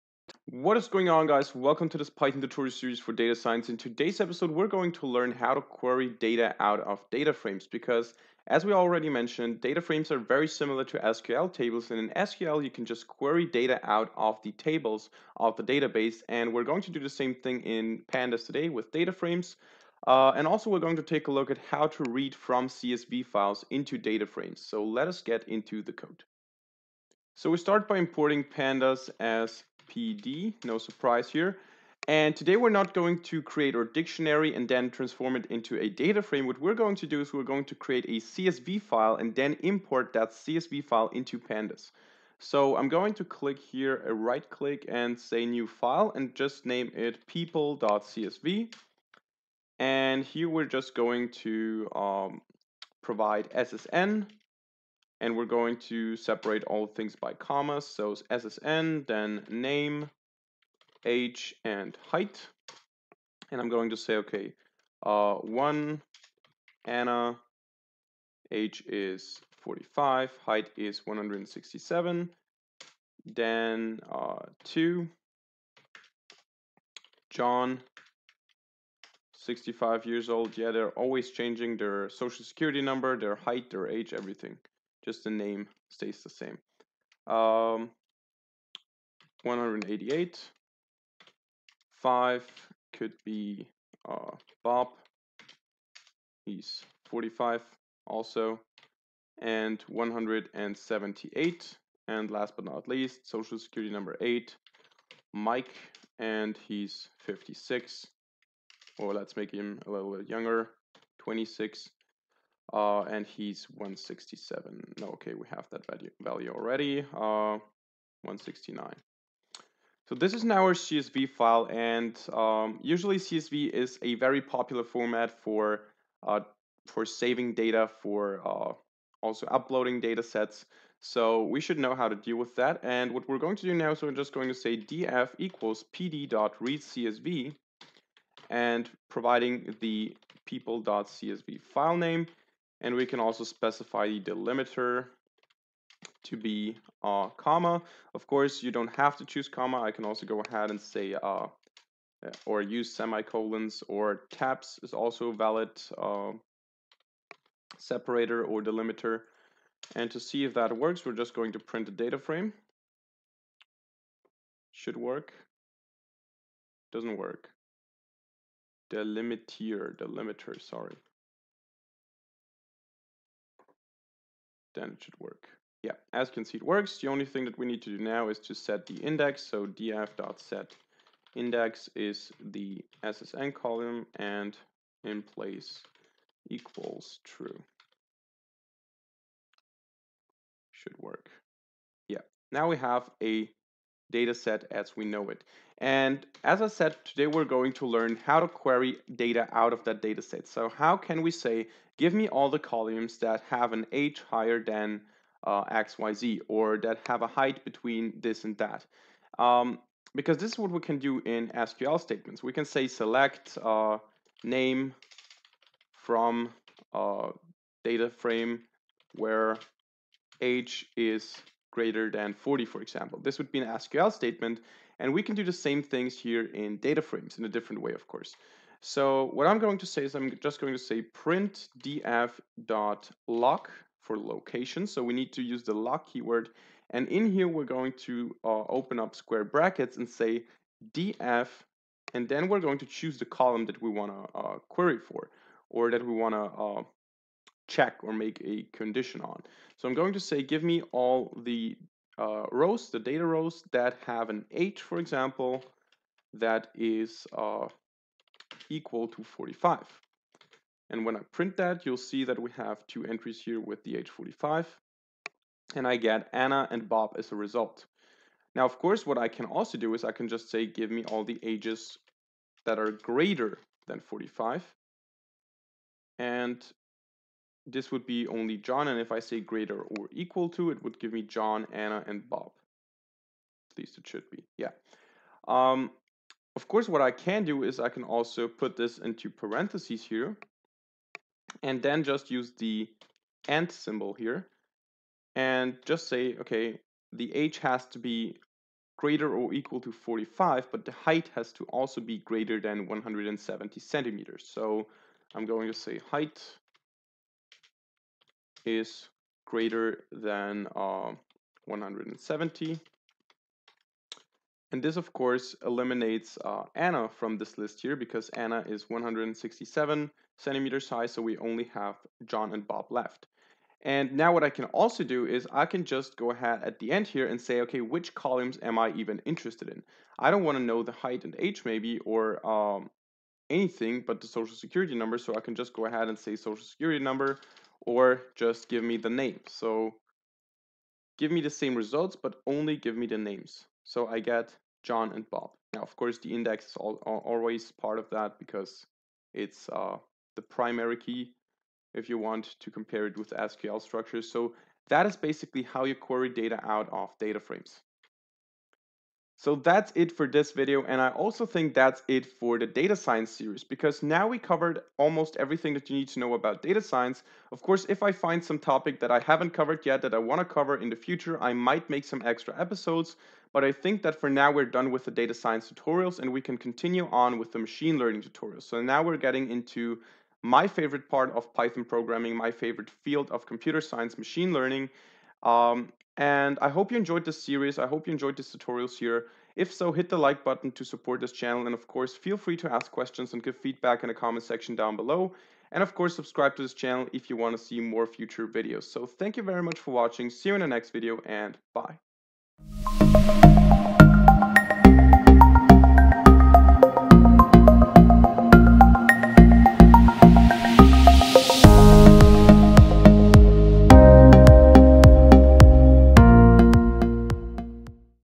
Python Data Science Tutorial #17 - Pandas Queries
In this video we are going to talk about querying data out of data frames with Pandas.